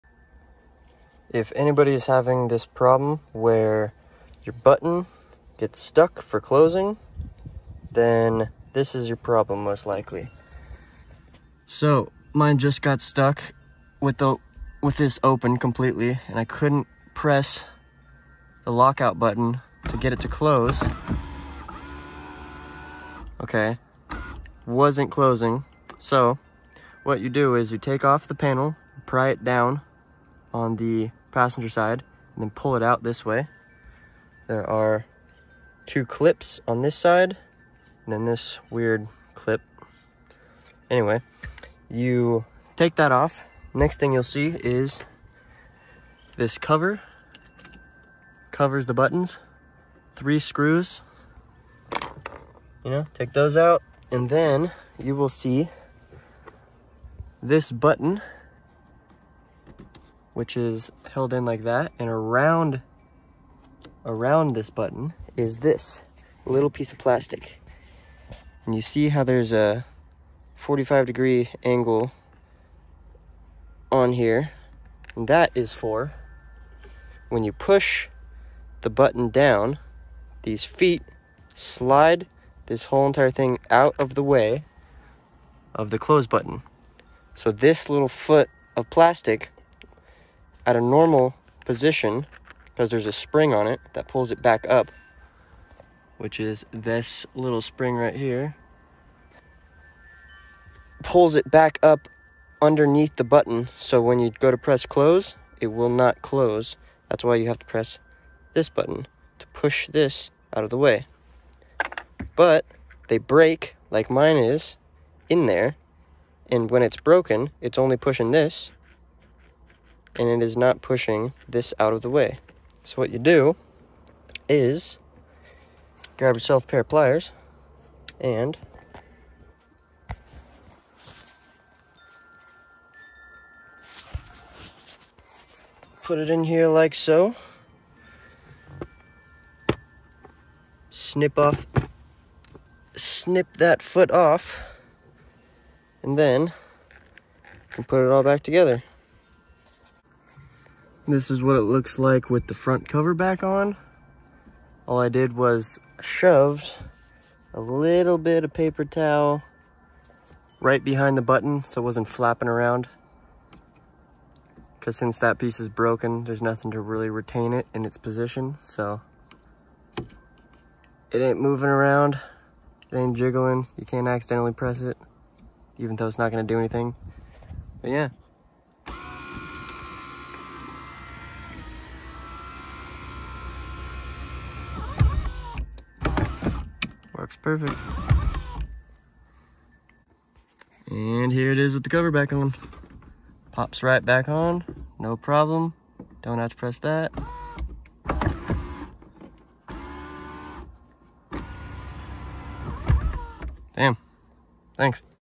1990-1995 2nd Gen Toyota 4Runner Sunroof Button Mod / Switch Fix
Quick and easy free method to fix your sunroof Switch￼  ￼